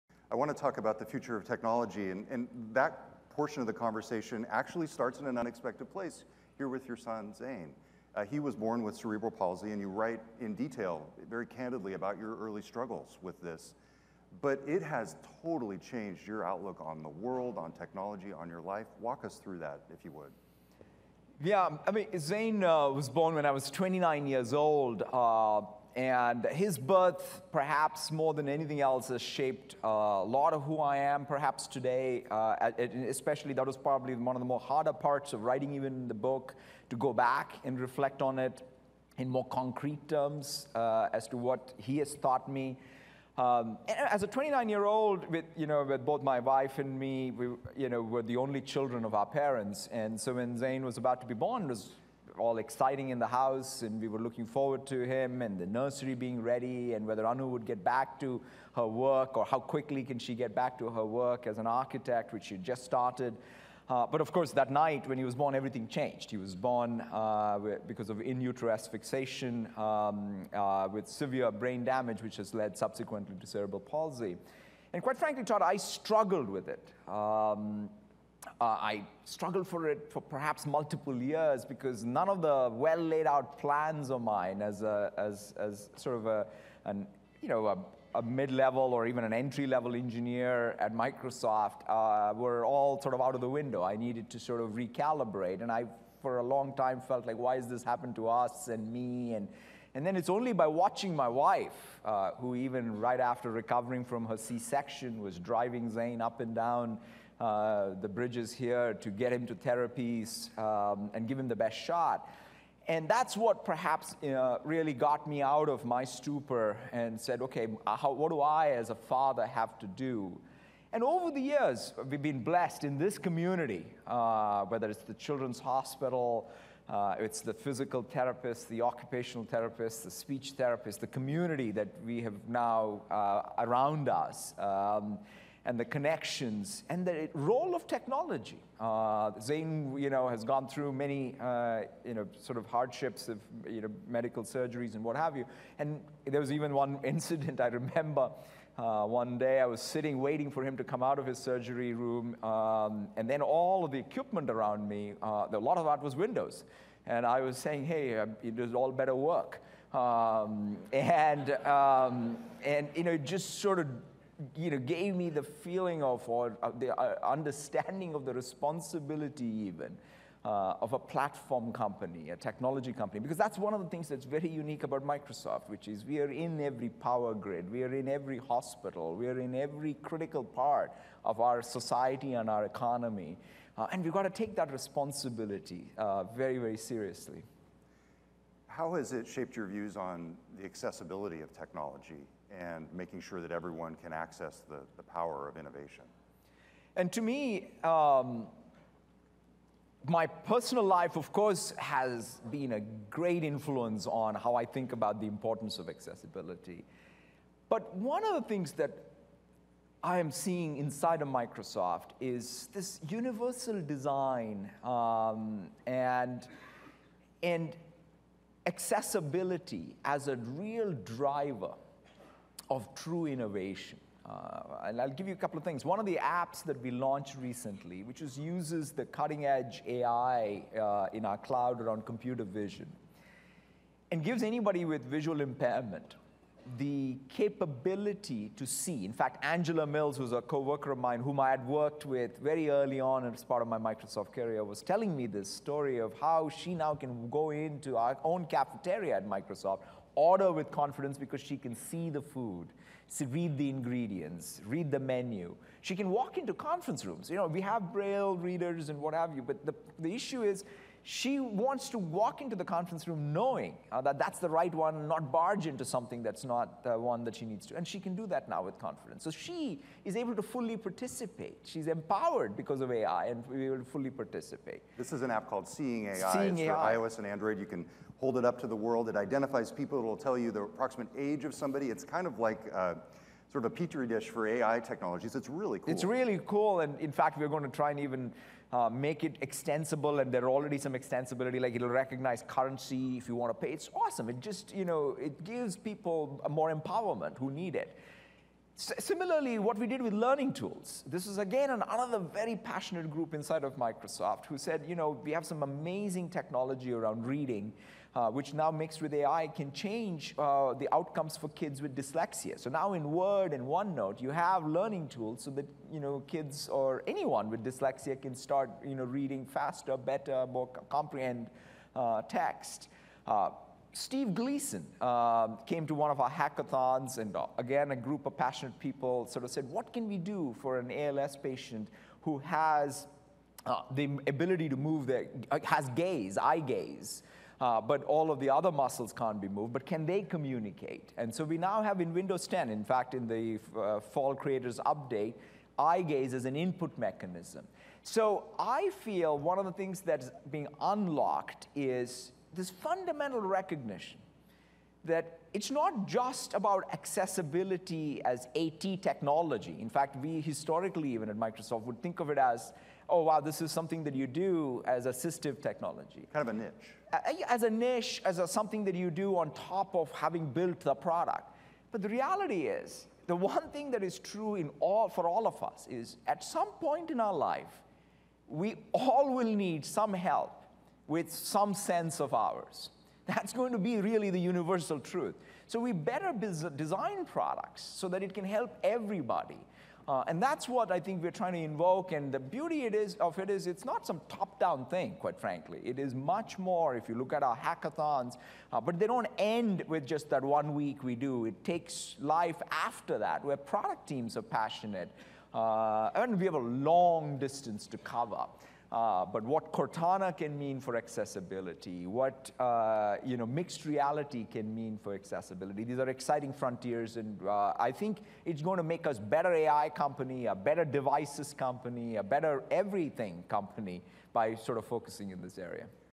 Satya Nadella, "Hit Refresh"
Microsoft CEO, Satya Nadella on the future of technology.